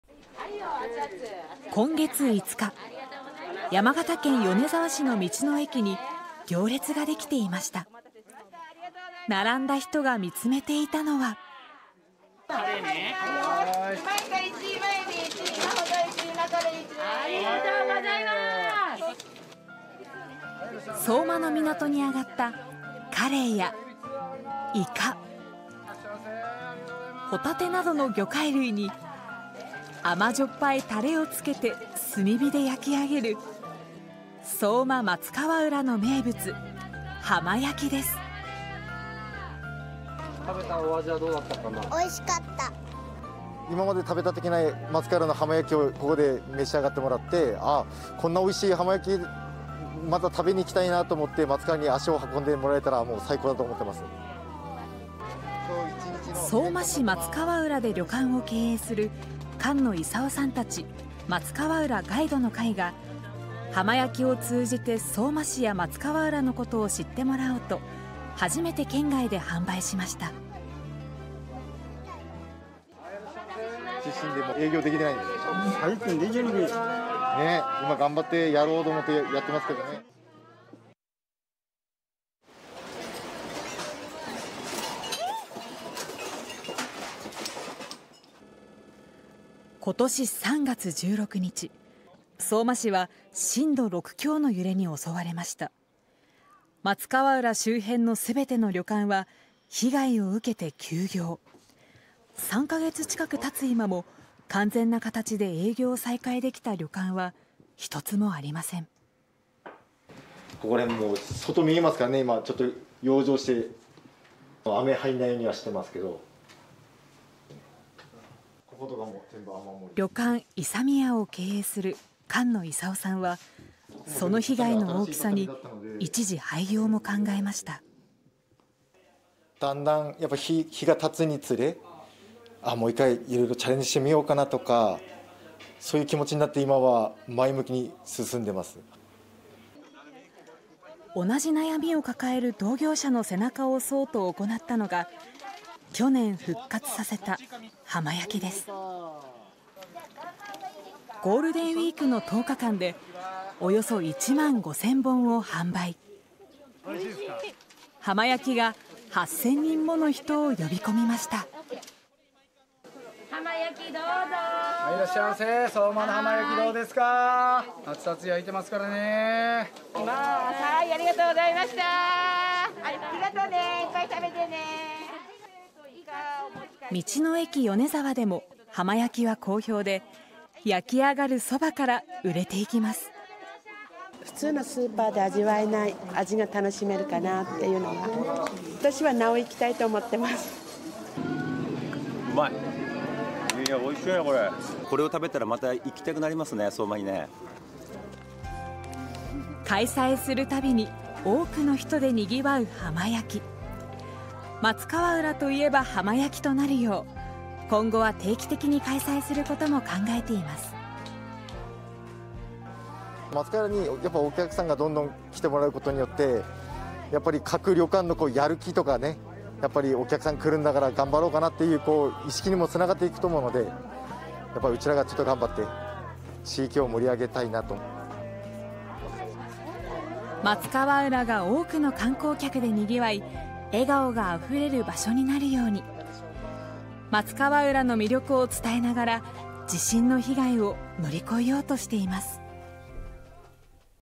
浜焼きの煙は復活ののろし 地震被害で傷ついた旅館街 地元の味が地域を救う【福島発】 (22/06/12 19:00)
２０２２年３月の地震で、福島県内では２万８０００棟余りの建物が被害を受けた。福島県相馬市松川浦の旅館は、一時すべてが休業し今も元の姿には戻っていない。そこで立ち上がったのが旅館の経営者たち。地域の味・浜焼きで、再びかつての賑わいを取り戻そうとしている。

６月５日、山形県米沢市の『道の駅』に行列が出来ていた。
並んだ人が見つめていたのは…福島県・相馬の港に揚がったカレイやイカ、ホタテなどの魚介類に甘じょっぱいタレをつけて炭火で焼き上げる、相馬松川浦の名物『浜焼き』

旅館いさみや・管野功さん：「今まで食べたことがない、松川浦の浜焼きをここで召し上がってもらって『こんな美味しい浜焼き、また食べに行きたいな』って思ってもらって松川浦に足を運んでもらえたら、もう最高だと思ってます」

福島県相馬市松川浦で旅館を経営する管野功さんたち『松川浦ガイドの会』が、浜焼きを通じて相馬市や松川浦のことを知ってもらおうと、初めて県外で販売した。

管野さん：「地震で営業できなくなっちゃって」
買い物客：「再建できんの？」
管野さん：「今やろうと思って頑張ってますけどね」

２０２２年３月１６日。福島県相馬市は震度６強の揺れに襲われた。
松川浦周辺のすべての旅館は被害を受けて休業。３カ月近く経っても完全な形で営業を再開できた旅館は一つもない。

『旅館いさみや』を経営する管野功さんは、その被害の大きさに一時廃業も考えた。
旅館いさみや・管野功さん：「だんだん日が経つに連れ、もう一回色々チャレンジしてみようかなとか、そういう気持ちになって今は前向きに進んでいます」

同じ悩みを抱える同業者の背中を押そうと行ったのが、２０２１年に復活させた『浜焼き』
ゴールデンウィークの１０日間で、約１万５０００本を販売。『浜焼き』が８０００人もの人を呼び込んだ。

『道の駅米沢』でも浜焼きは好評で、焼きあがるそばから売れていった。

買い物客：「普通のスーパーでは味わえない味が楽しめるかなっていうのが。（浜焼きを食べて）私は尚、松川浦に行きたいと思ってます」
買い物客：「うまい！いや～美味しいなこれ！これを食べたら、また行きたくなりますね、相馬にね」

開催するたびに、多くの人で賑わう『浜焼き』
「松川浦と言えば浜焼き」となるよう、今後は定期的に開催することも考えている。

旅館いさみや・管野功さん：「松川浦にお客さんがどんどん来てもらうことによって、各旅館のやる気とか『お客さん来るんだから頑張ろうかな』っていう士気にも繋がっていくと思うので、ウチらがちょっと頑張って地域を盛り上げたいなと」

松川浦が、多くの観光客で賑わい笑顔が溢れる場所になるように。
松川浦の魅力を伝えながら、地震の被害を乗り越えようとしている。